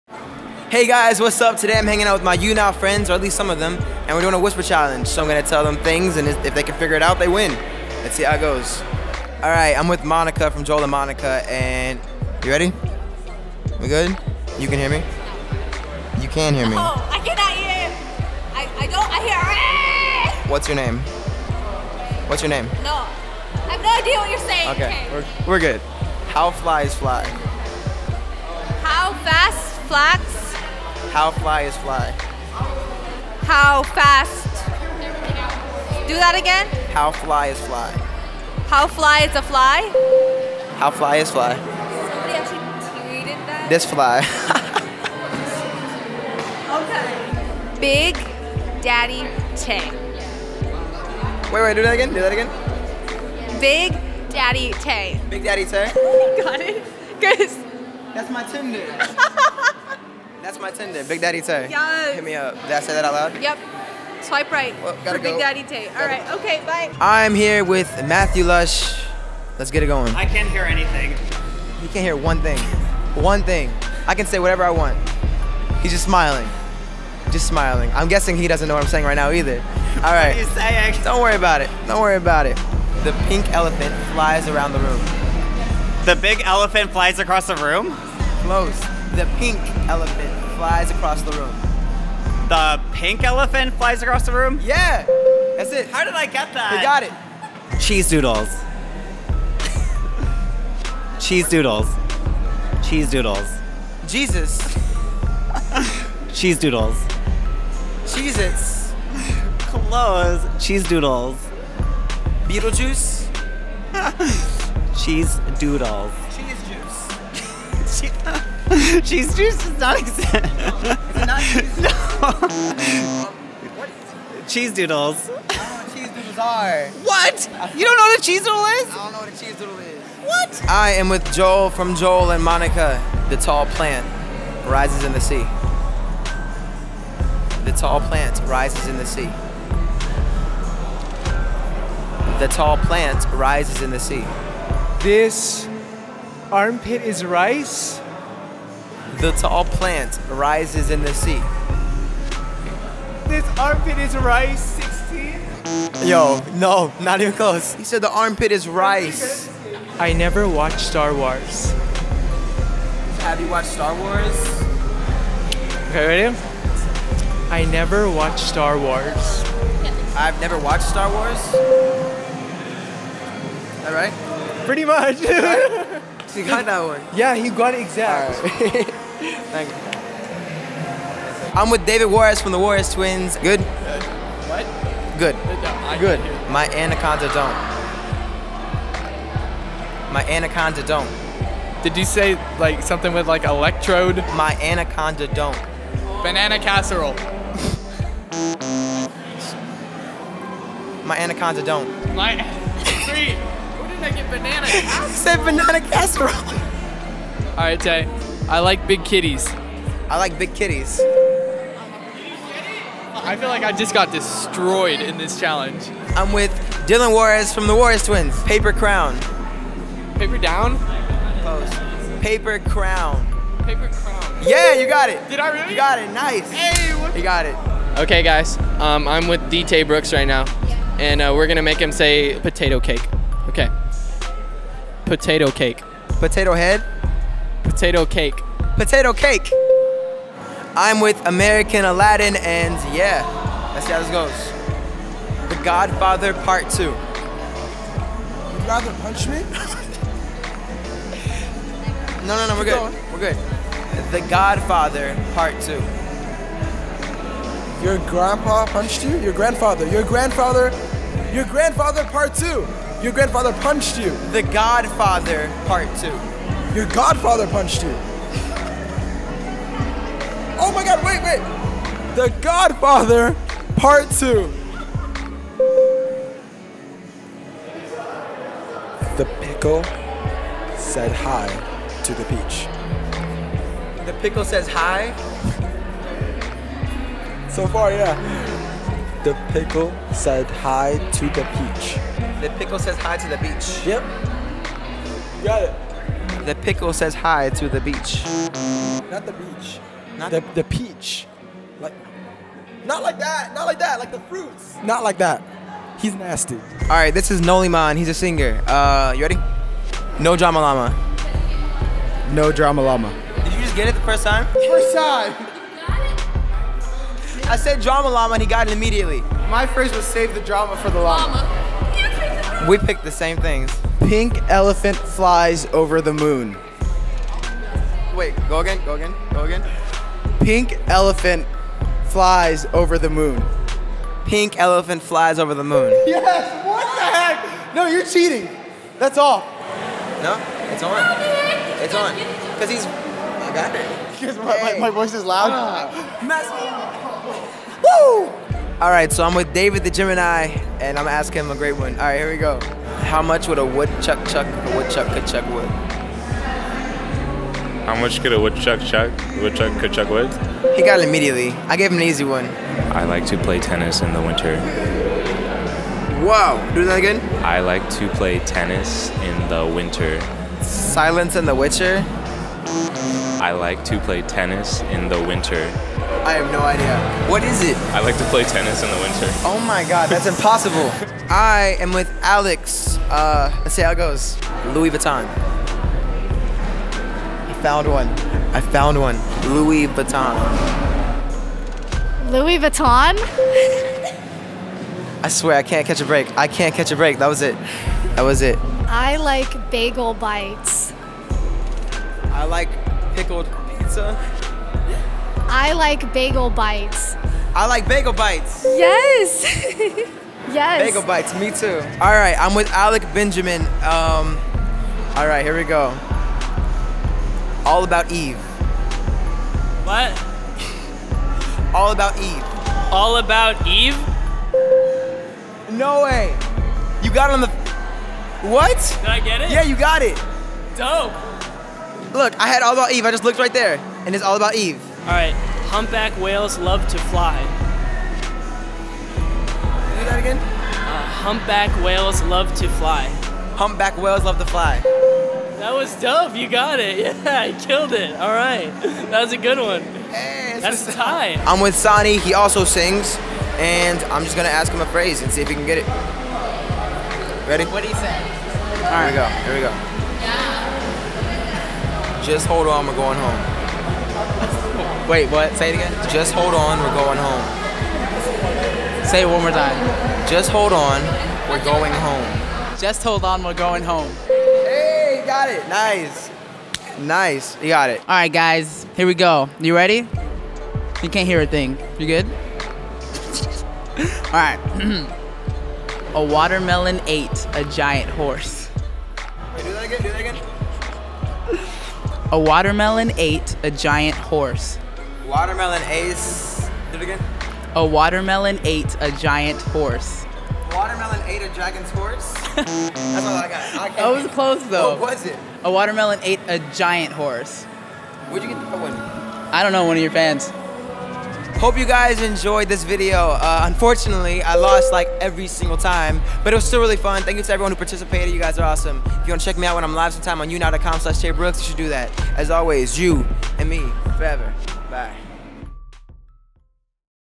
WHISPER CHALLENGE
I had a chance to hangout with some of my YouNow fam and we did the Whisper Challenge! Watch to the end to see who wins!

Be sure to stop by my friends' broadcasts on YouNow and tell them I love them (Especially Dillon)!

♥ ShawTaes